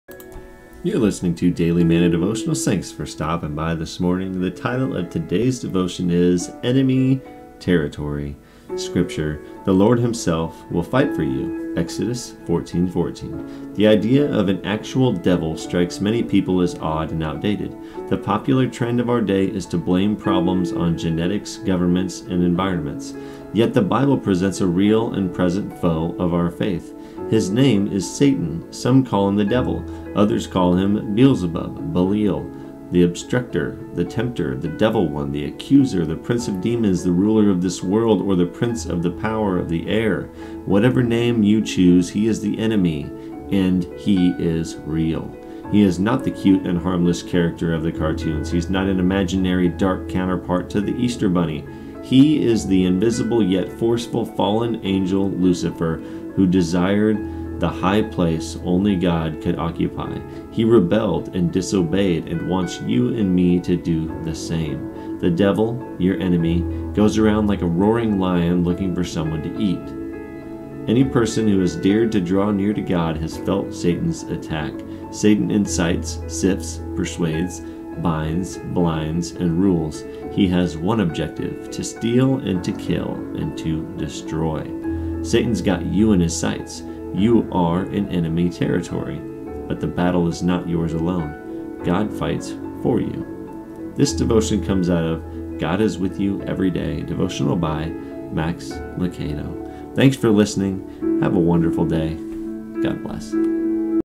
Daily Manna Devotional - Enemy Territory (Max Lucado) 7/17/21
ENEMY TERRITORY

Scripture: “The Lord himself will fight for you.” (Exodus 14:14 NLT)

The idea of an actual devil strikes many people as odd and outdated. The popular trend of our day is to blame problems on genetics, governments, and environments. Yet the Bible presents a real and present foe of our faith. His name is Satan. Some call him the Devil. Others call him Beelzebub, Belial, the obstructer, the tempter, the devil one, the accuser, the prince of demons, the ruler of this world, or the prince of the power of the air. Whatever name you choose, he is the enemy, and he is real.
He is not the cute and harmless character of the cartoons. He is not an imaginary, dark counterpart to the Easter Bunny. He is the invisible yet forceful fallen angel Lucifer, who desired the high place only God could occupy. He rebelled and disobeyed and wants you and me to do the same. “The devil, your enemy, goes around like a roaring lion looking for someone to eat” (1 Peter 5:8 NCV)
Any person who has dared to draw near to God has felt Satan’s attack. Satan incites, sifts, persuades, binds, blinds, and rules. He has one objective: “to steal, and to kill, and to destroy” (John 10:10). Satan’s got you in his sights. You are in enemy territory. But the battle is not yours alone. God fights for you.

This devotion comes out of, “God Is With You Every Day,” Devotional, by Max Lucado